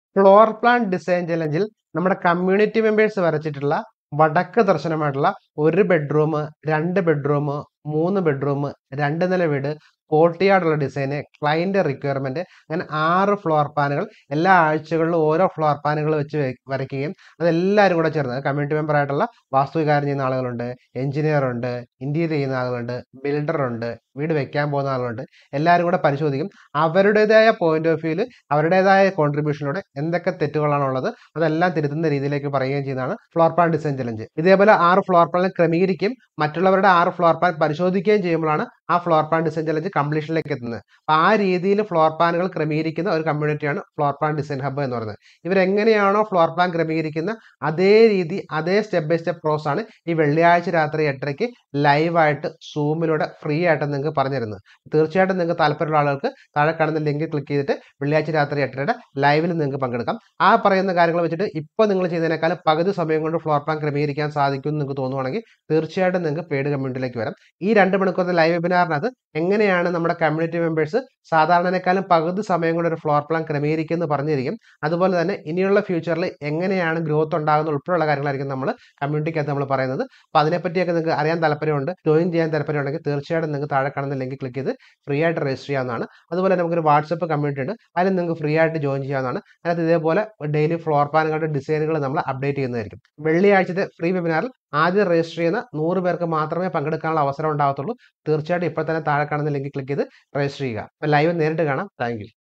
Free Join Kerala's First Vastu & Space Optimized Floor Plan Design Live Intractive Master Class
Free Join Kerala's First Vastu & Space Optimized Floor Plan Design Live Interactive Master Class (2 HOUR)

🔒  SECRET -1
Budjet Sq Feet Area അനുസരിച്ചു വാസ്തു പ്രകാരം ഉള്ള ഭിത്തിപ്പുറം ചുറ്റളവ് Easy ആയി എടുക്കുന്ന വിധം

🔒  SECRET -2
ദർശനത്തിനു അനുസരിച്ചു മുറികളുടെ Position വാസ്തുവും Space Optimization കൂടി ഉൾപ്പെടുത്തി Design ചെയ്യുന്ന വിധം.

🔒  SECRET -3
മുറികളുടെ വാസ്തു അളവുകൾ Sq. Feet Budget അനുസരിച്ചു തുടക്കക്കാർക്ക് വരെ എങ്ങനെ Easy ആയി ഇടാം.

NOTE : ONLY ONE TIME : FIRST REGISTRATION & class  പൂർണമായും attend  ചെയ്യുന്ന 20 ആളുകൾക്ക് 15  minute  one  to  one free  meeting (within 7 days) അവസരം ഉണ്ടായിരിക്കും.

Free Register Next Live Webinar

Join this WhatsApp community for free Registration links & Floor Plan Design